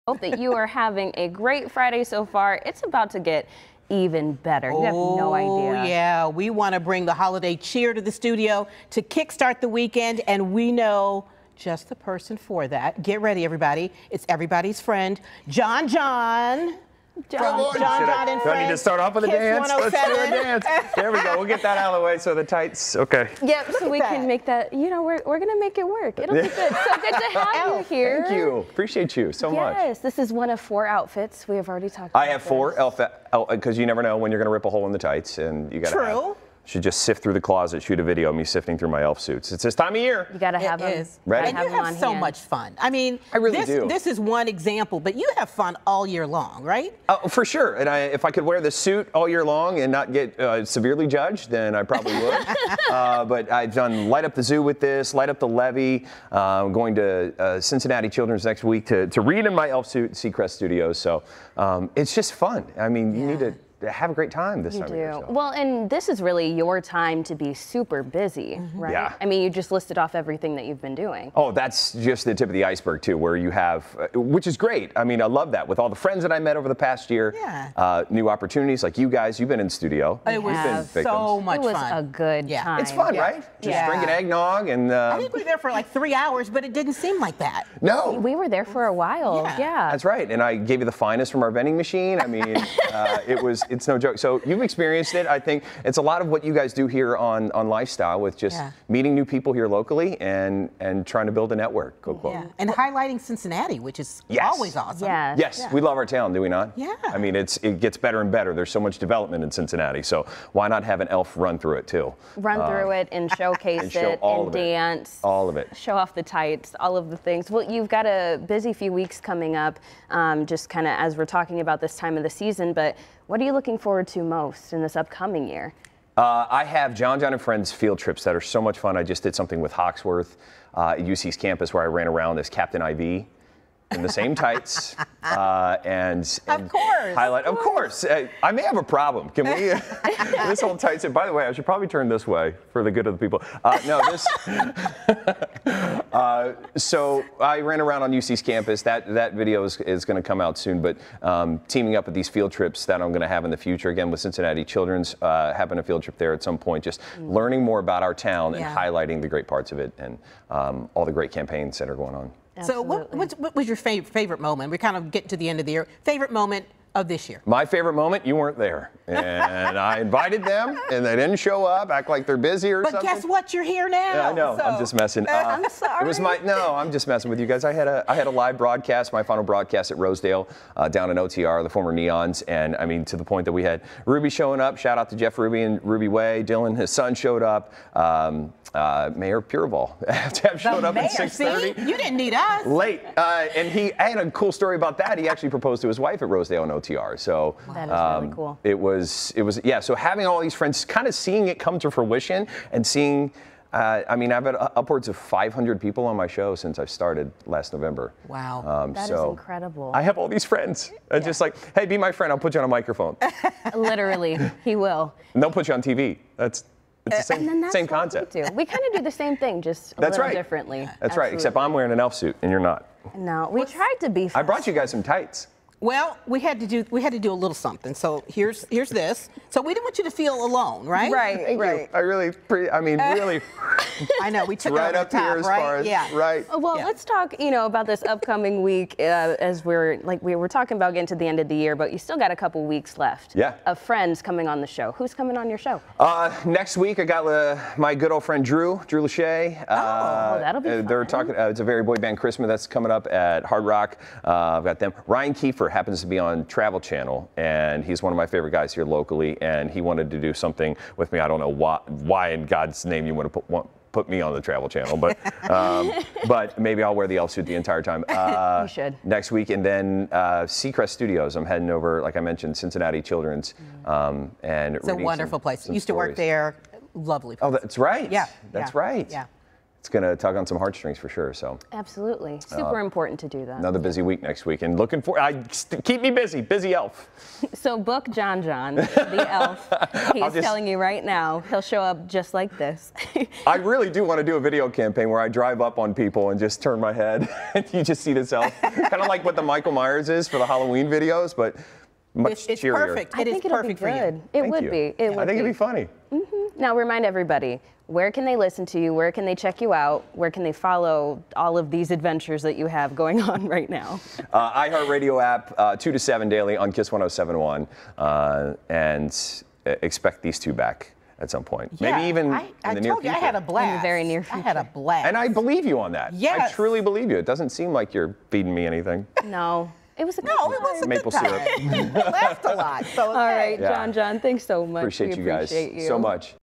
JonJon visits Cincy Lifestyle for the holidays
When Kiss107's JonJon stops by, you know it's going to be a good time! We caught up with him to talk about a year of JonJon and Friends and what he has planned for the rest of this festive season.
Grab some candy and syrup for this one.

Listen to JonJon and Friends from 2-7pm every weekday on iHeartRadio!